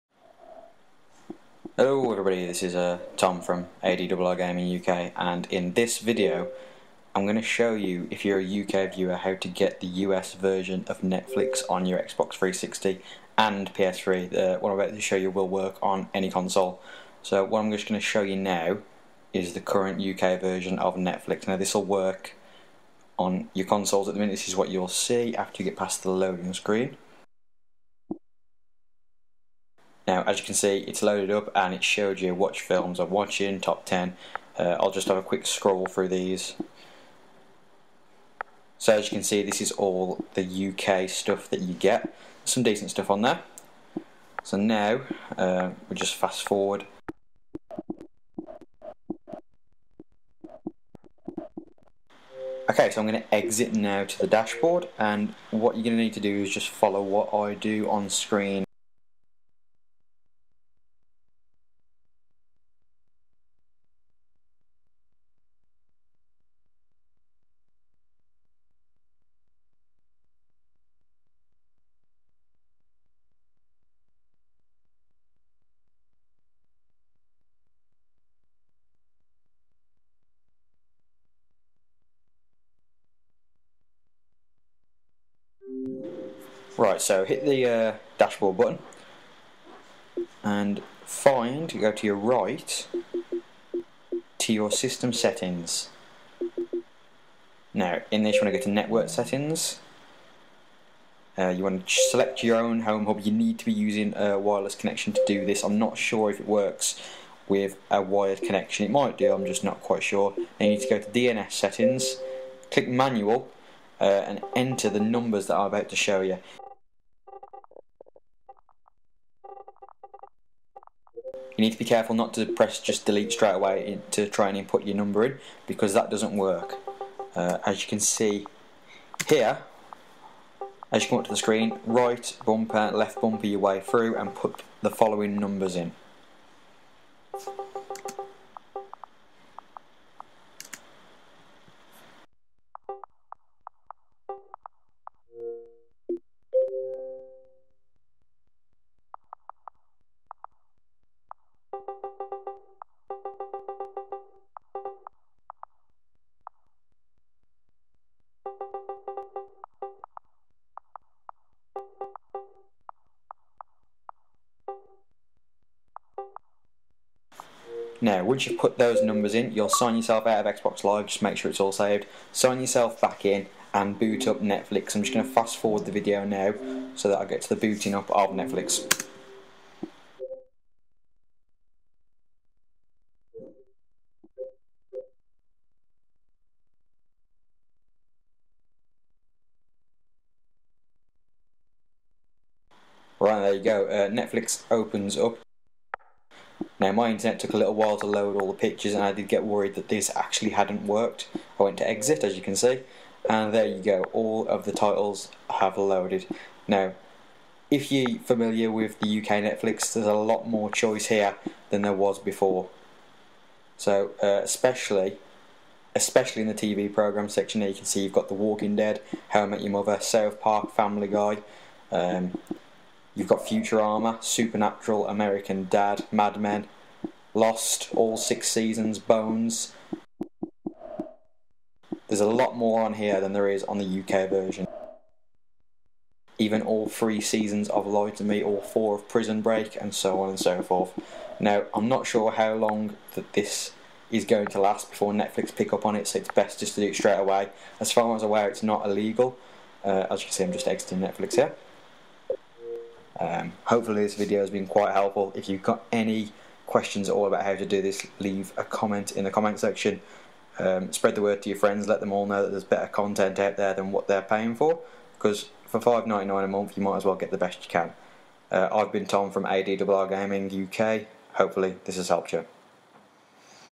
How to get US Netflix Programmes on a UK Xbox 360
This method works on both PS3 and Xbox 360.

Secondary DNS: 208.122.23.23